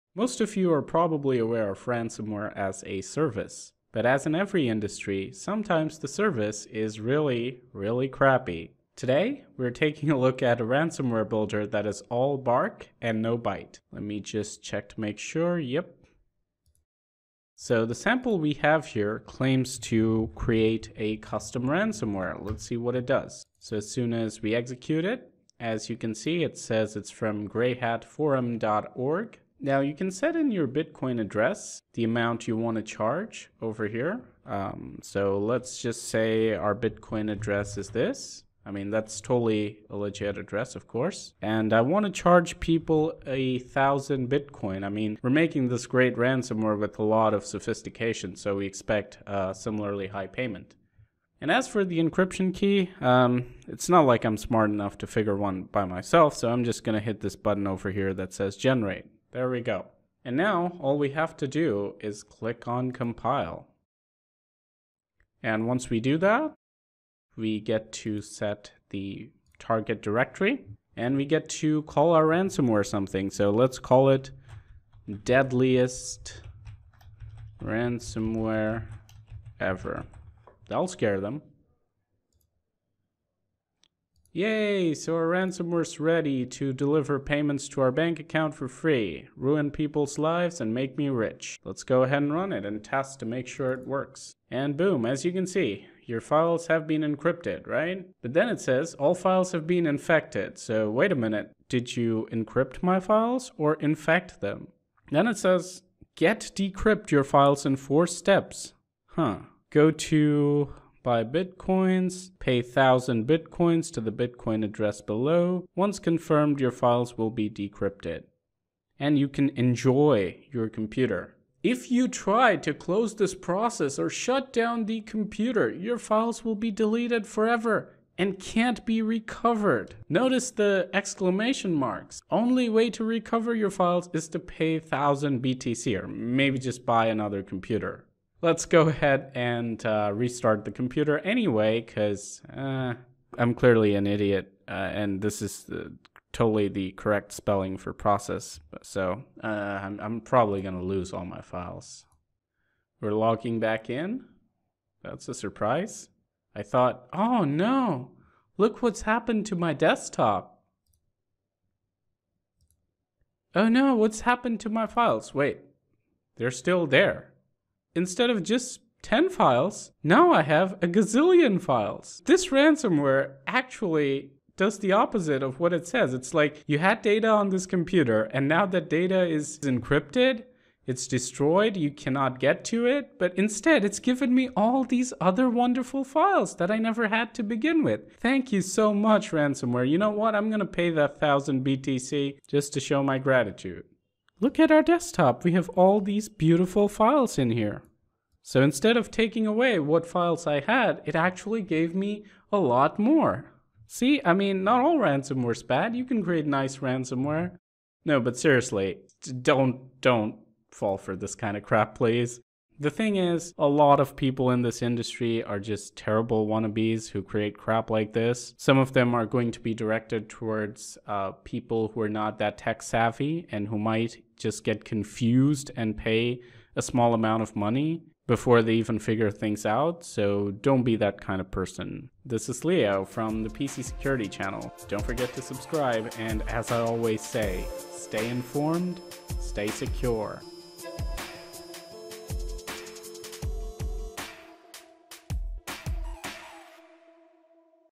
Pathetic Ransomware Builders | Empty Threats
Ransomware as a Service (RaaS) has been a major annoyance for a while, but most builders create broken and empty threats meant to extort bitcoins behind the guise of encryption, that is actually non-existent. This video highlights such a sample, which I encountered recently.